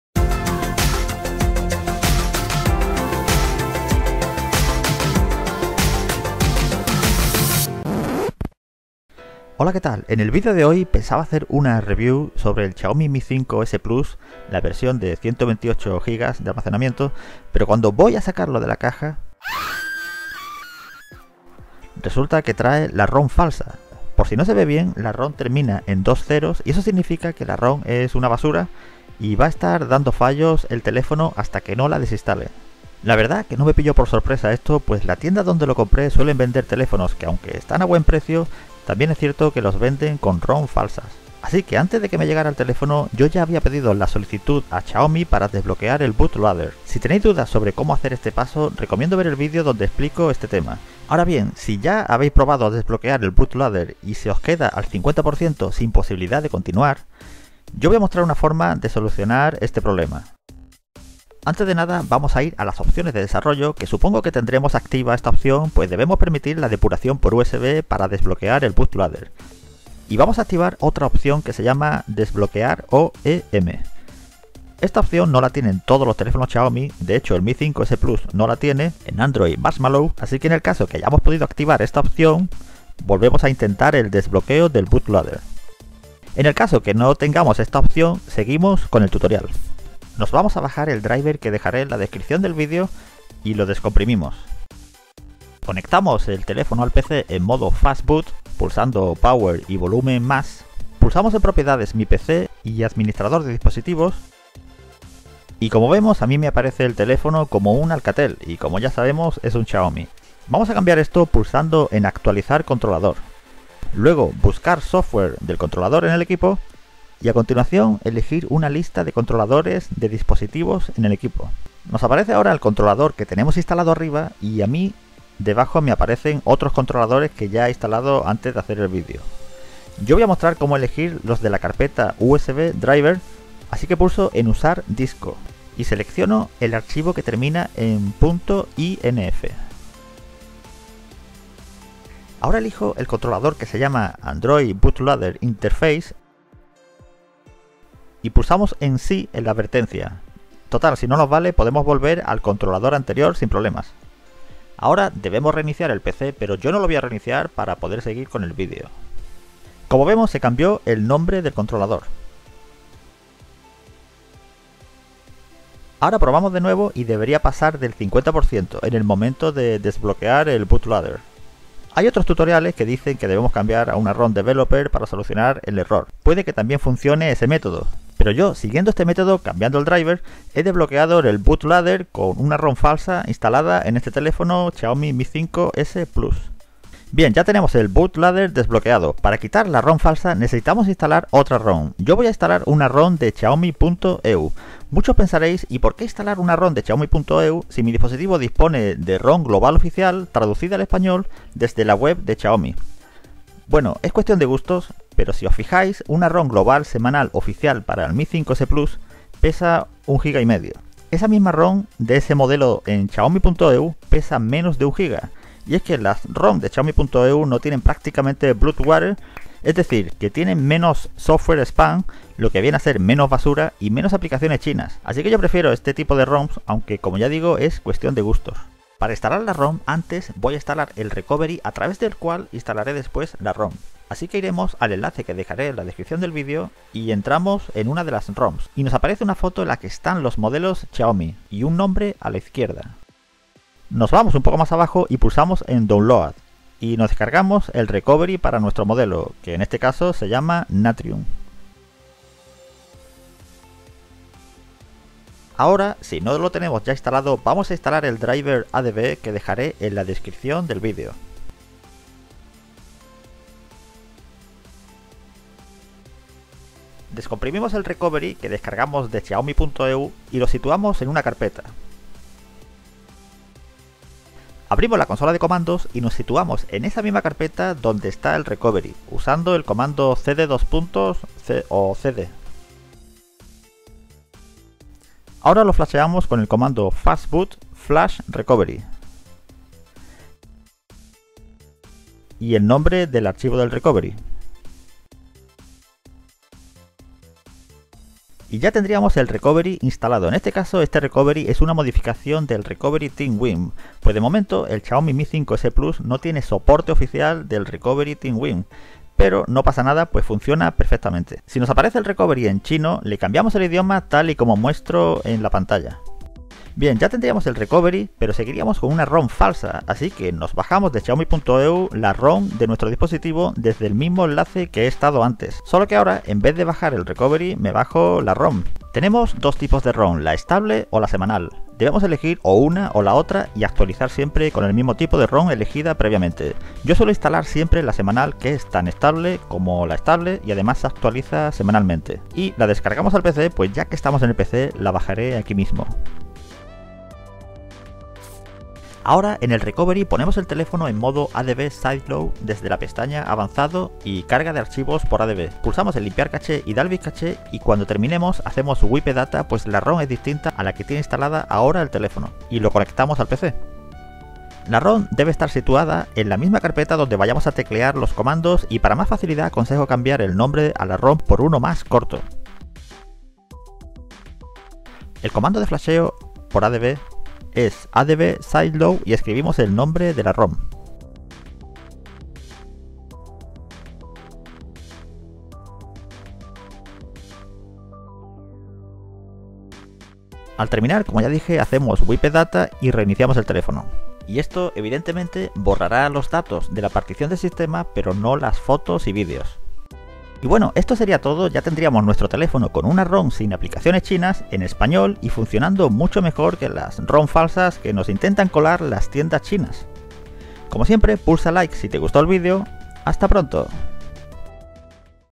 Quitar Rom Falsa Xiaomi + Solucionar error 50% Unlock Bootloader
Como solucionar el error del 50% cuando desbloqueas el bootloader con miui unlock y luego como cambiar la rom falsa por una de Xiaomi.eu.
Este método es recomendable hacerlo por lo sencillo que es pero en caso que no funcione recomiendo cambiar la ROM via EDL desde este enlace explico como y volver a pasarle de nuevo miunlock si queremos desbloquear el bootloader